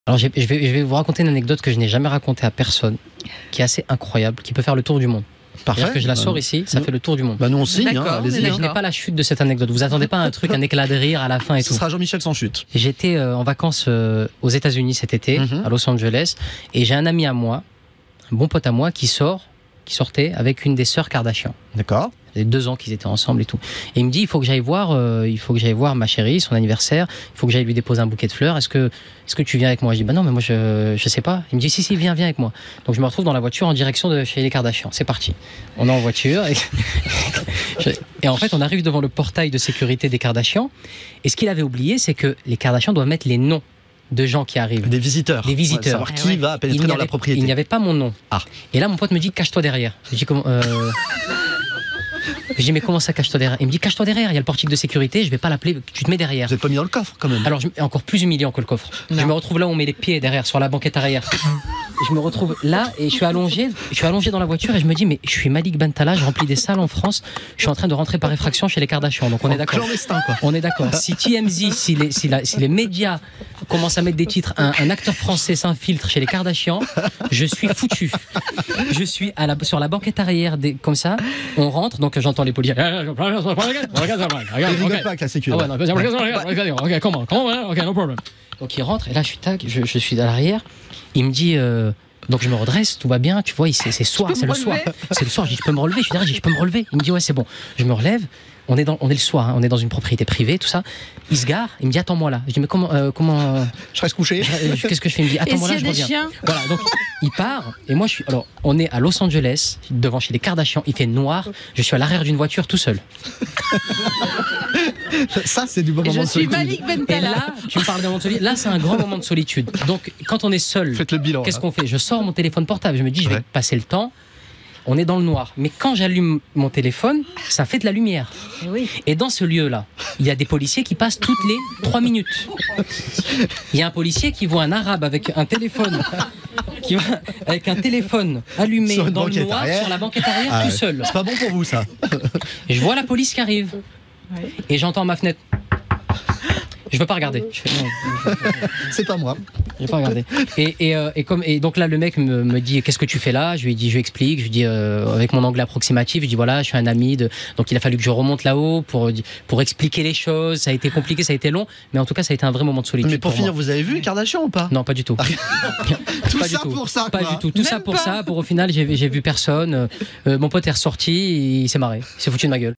Malik Bentalha : Son hilarant moment de solitude chez les Kardashian
Malik Bentalha raconte le jour où il s'est infiltré chez la star américaine...